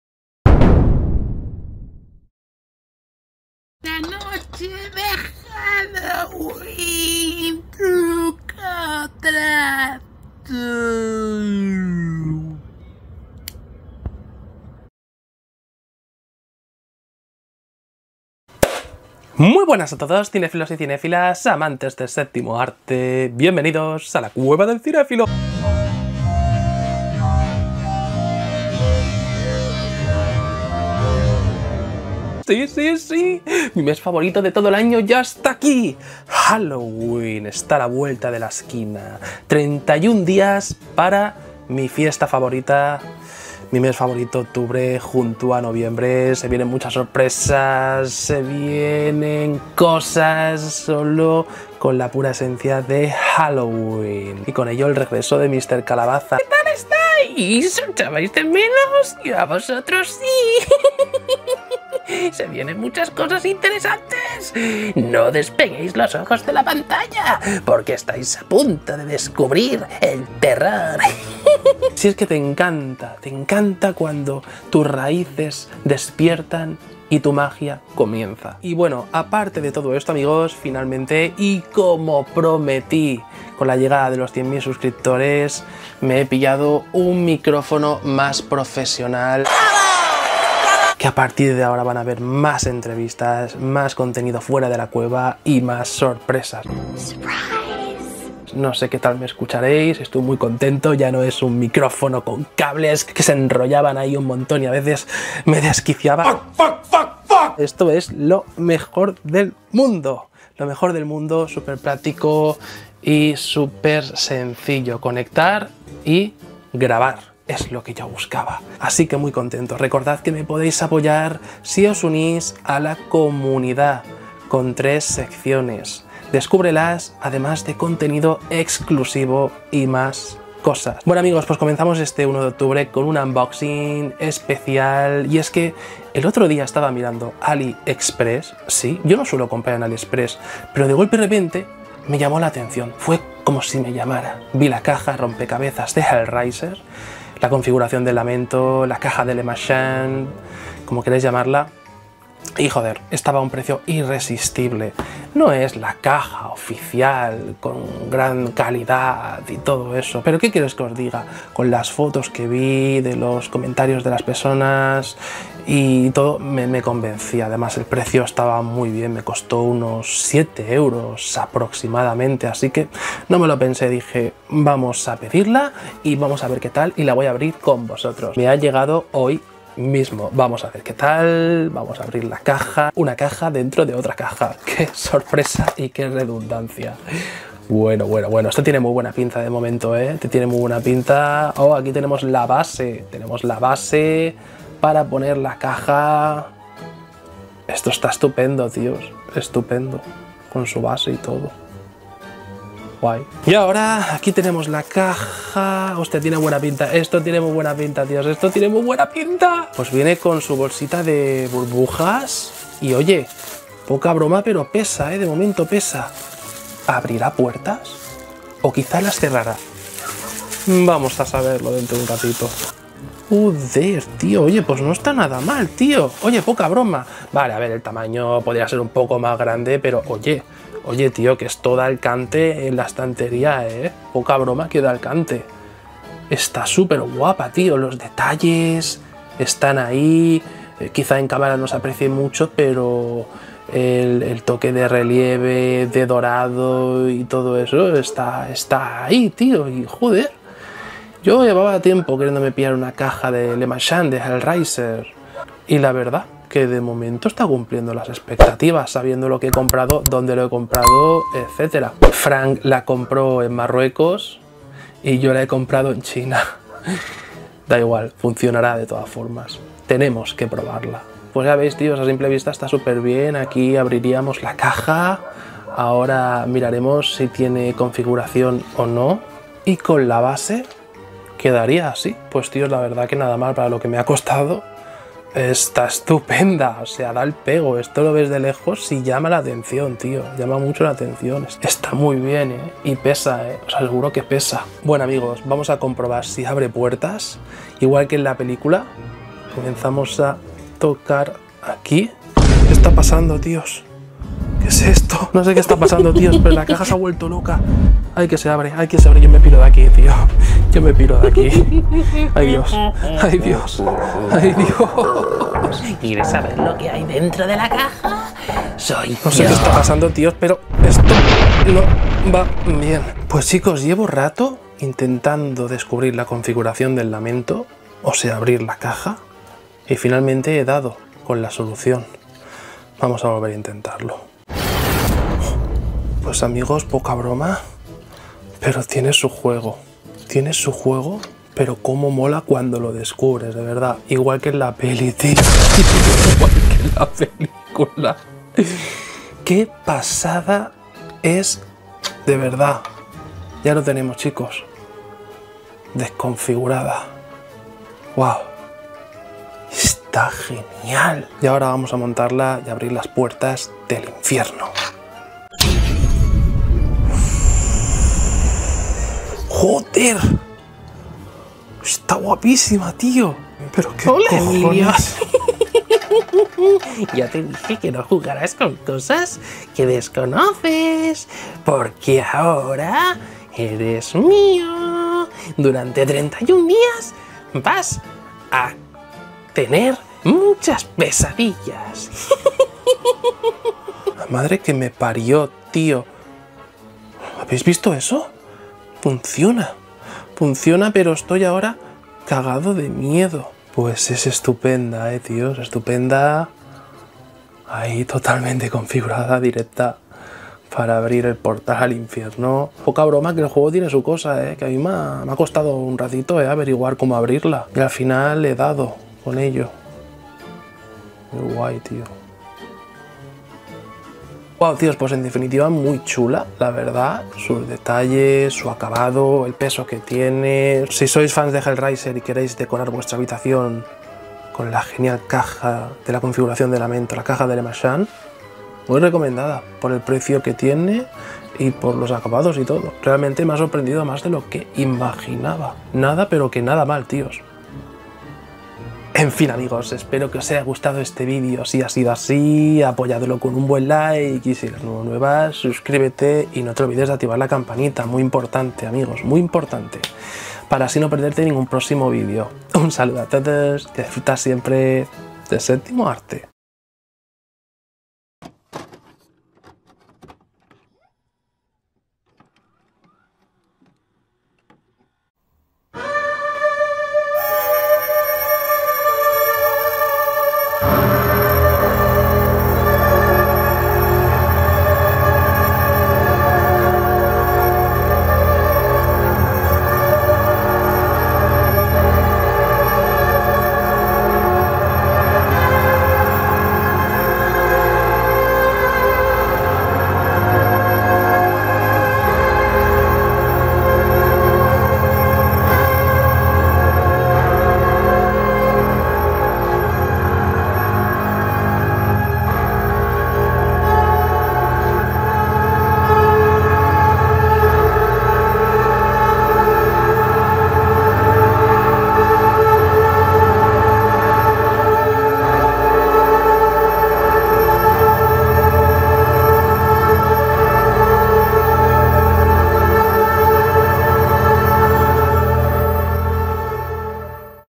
TEMPORADA de HALLOWEEN con la CAJA ROMPECABEZAS 🔥 HELLRAISER 👀 UNBOXING
Descubre la genial caja rompecabezas de "Hellraiser" de AliExpress con un misterioso secreto y el regreso de Mr. Calabaza resucitado desde el mismísimo infierno con la locura, la diversión y la emoción. Novedades y más cosas para esta gran temporada llena de mucho, mucho terror.

Mi mes favorito ya está aquí 🎃